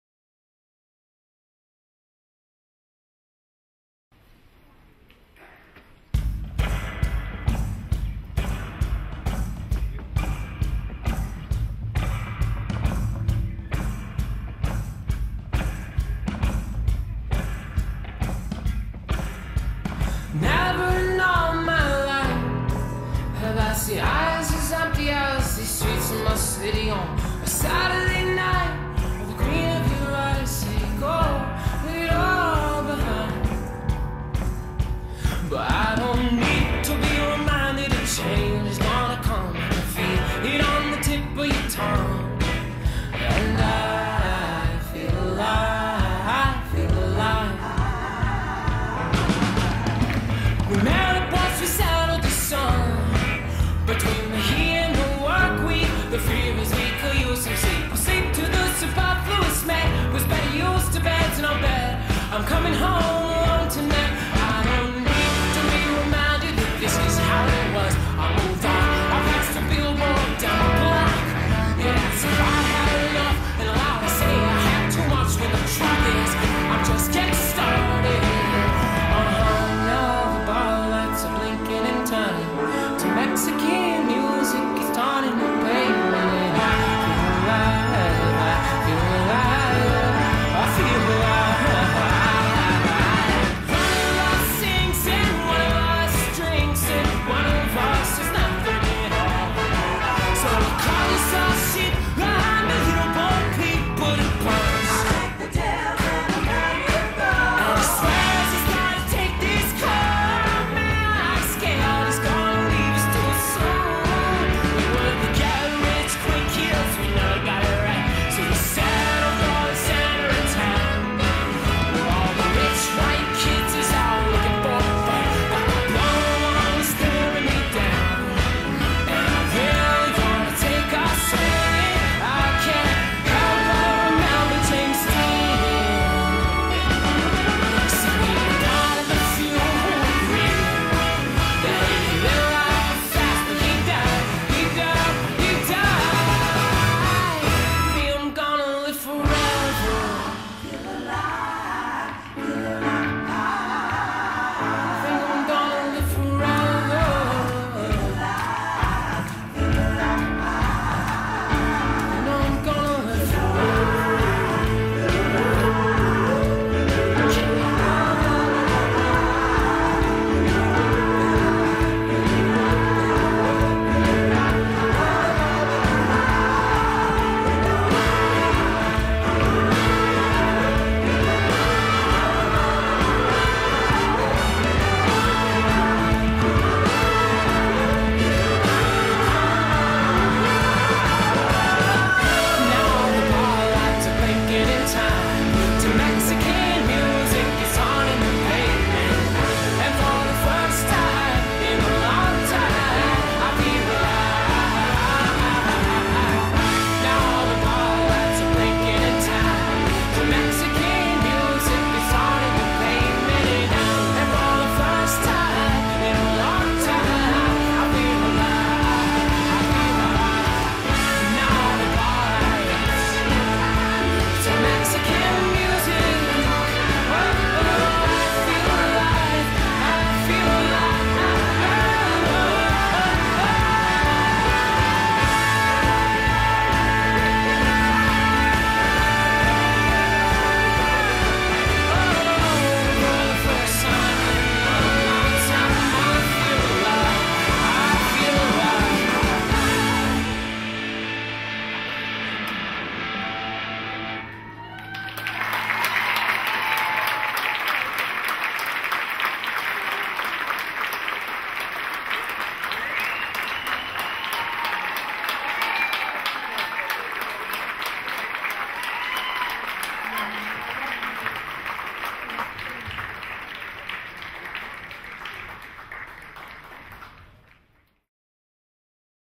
Saturday Night Hobos (Advance Tap) - 2013 Susan Barnes Dance Recital, Afternoon Performance
This video is from the 2013 Susan Barnes Dance Recital Afternoon Performance.  It features students from the Advanced Tap class.

For more information about Willow Street Studios, home of Susan Barnes Dance